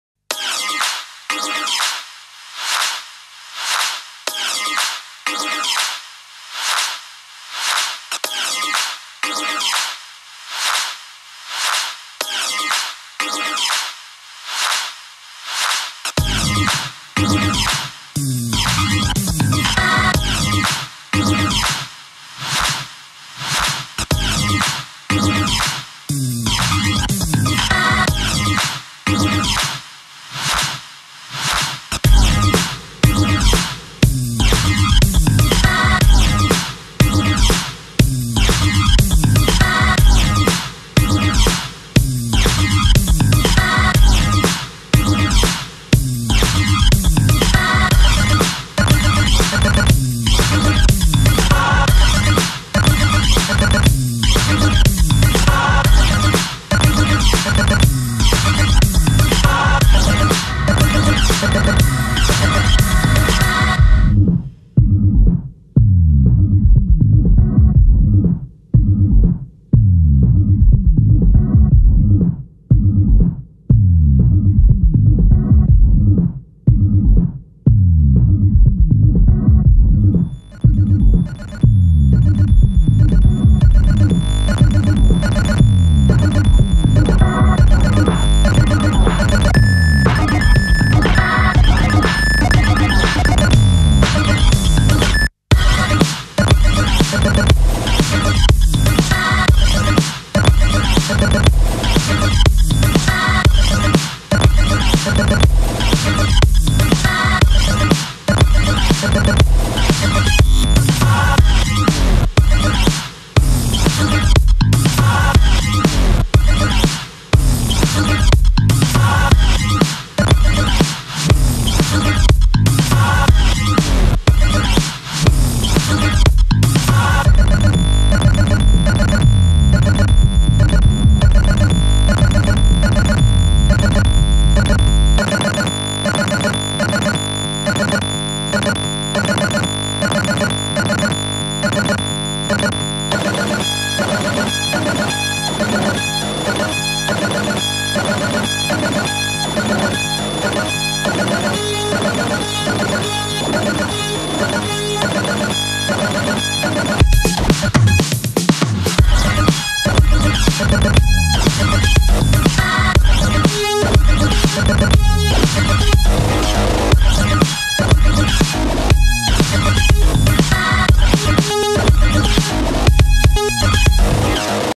Papercase
A movie made in the Reykjavik Academy of Arts(Iceland). Lokverkefni í stuttmyndagerð í fornámi myndlistaskólans. Hudreds of pieces of card-board cut and glued together! enjoy